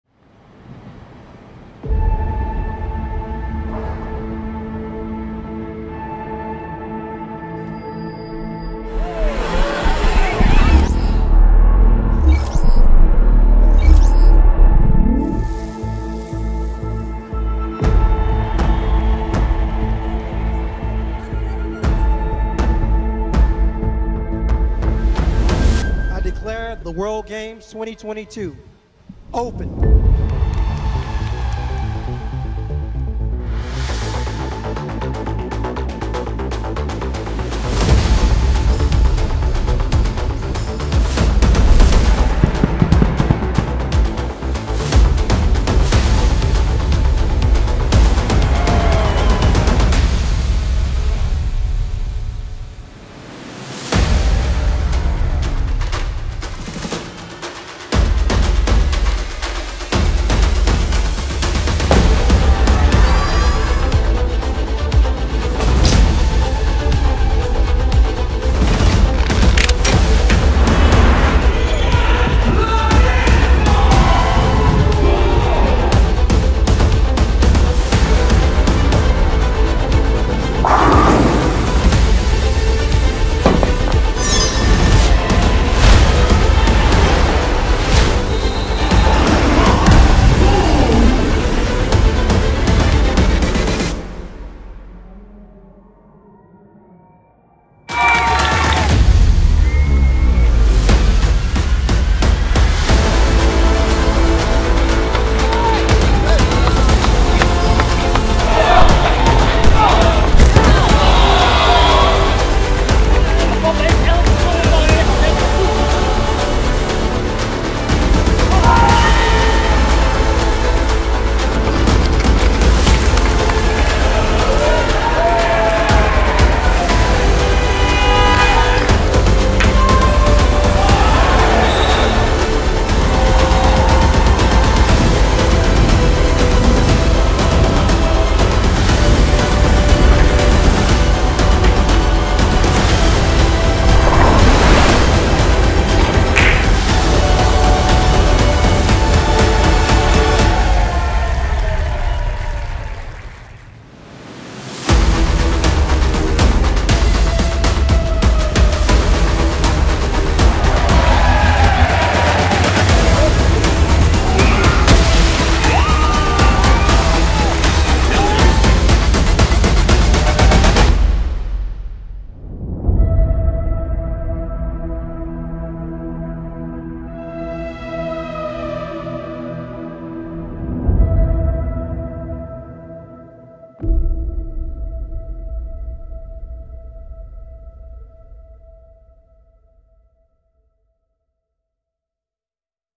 TheWorldGamesNL 2022 Aftermovie
Wat een heerlijke editie van de World Games in Birmingham. Wil je nog één keer de sfeer ervaren? Check dan nog even de aftermovie.